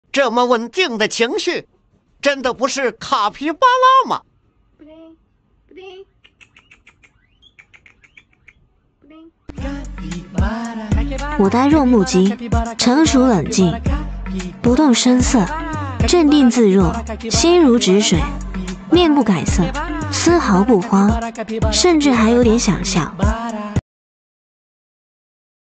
Unbelievably Calm Emotions! Are You Ready for an Incredible Journey? DutchPig GuineaPig...
Get ready for an extraordinary journey as we embark on an incredible adventure with these unbelievably calm emotions of Dutch Pig and Guinea Pig! Witness the cuteness overload and be amazed by their serene demeanor.